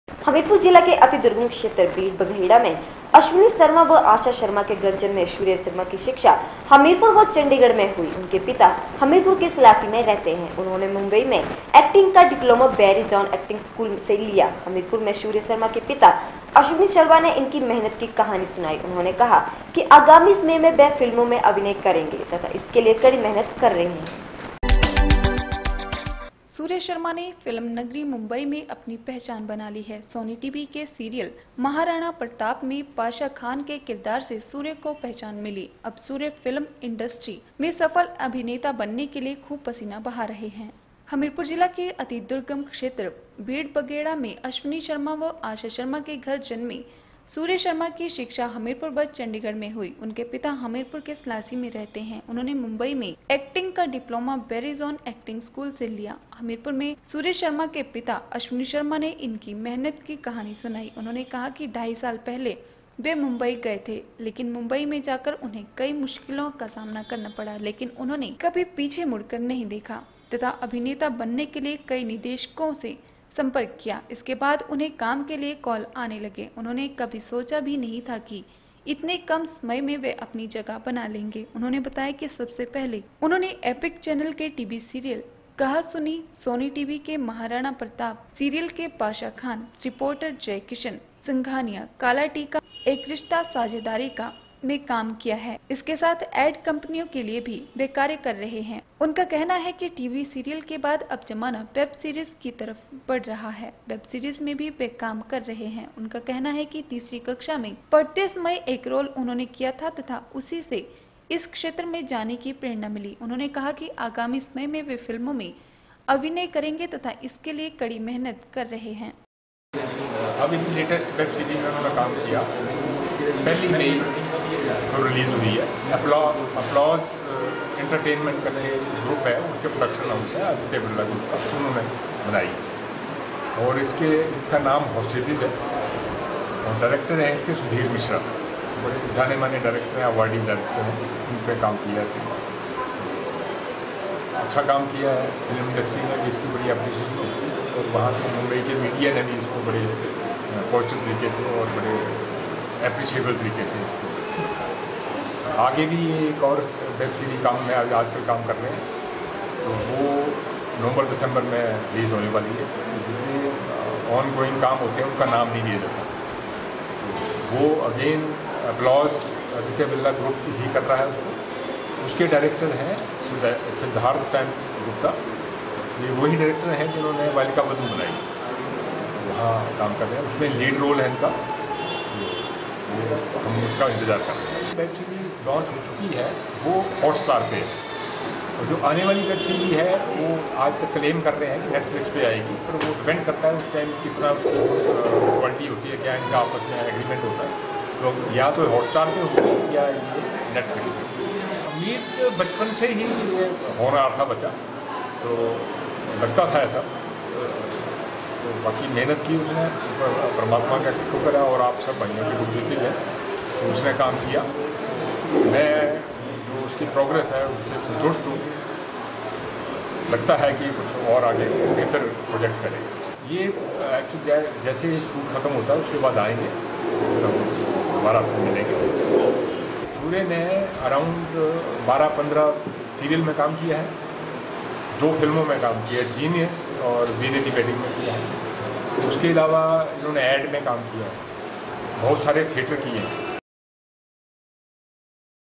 26 JUNE N 6 SuRYA Sharma's father AsWani Sharma in Hamirpur narrated the story of his hard work
हमीरपुर जिला के अति दुर्गम क्षेत्र बीड़ बगेहड़ा में अश्वनी शर्मा व आशा शर्मा के घर जन्मे सूर्य शर्मा की शिक्षा हमीरपुर व चंडीगढ़ में हुई। उनके पिता हमीरपुर के सलासी में रहते हैं। उन्होंने मुंबई में एक्टिंग का डिप्लोमा बैरी जॉन ए¨क्टग स्कूल से लिया। हमीरपुर में सूर्य शर्मा के पिता अश्वनी शर्मा ने इनकी मेहनत की कहानी सुनाई।